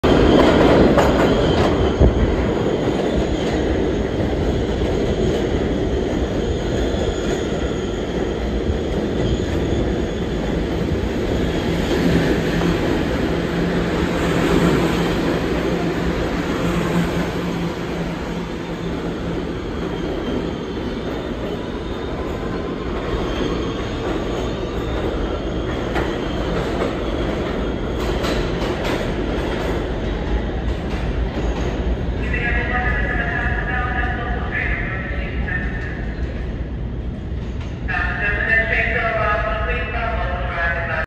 *just casually catches the new R211 set being delivered at hoyt-schermerhorn*
taken today on this day and the date that this was posted